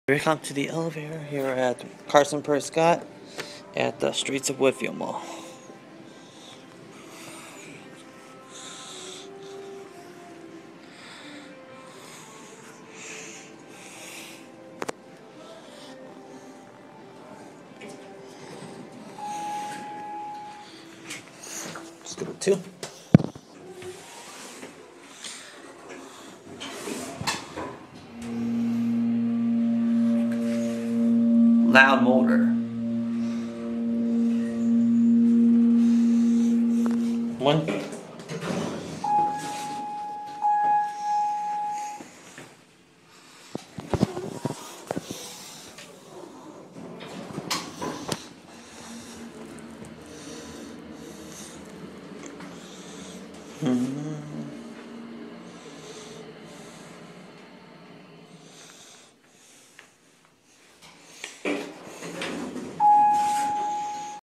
Graeginator Rides The Elevator at Carson Pirie Scott at Streets of Woodfield Mall in Schaumburg, IL
Carson Pirie Scott Streets Of Woodfield Mall in Schaumburg Illinois.